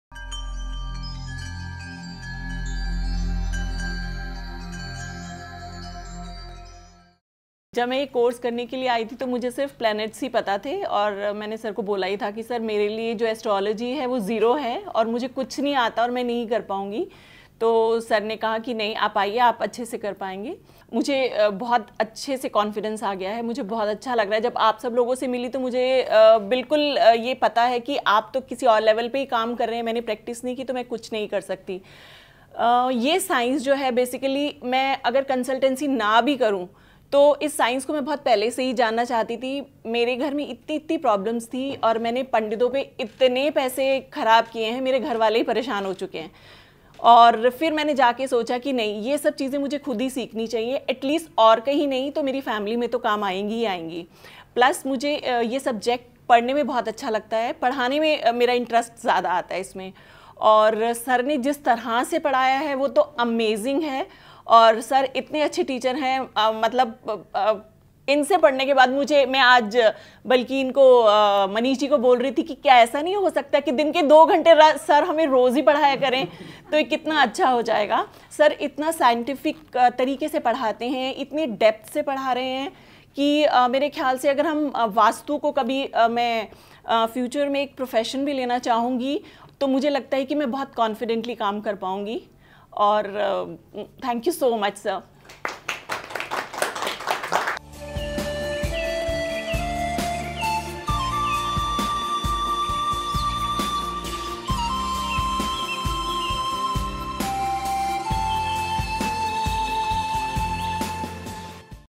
Sangeeta Jain ji feels everyone should do KundliWay Course
Sangeeta Jain ji shares her Course Experience.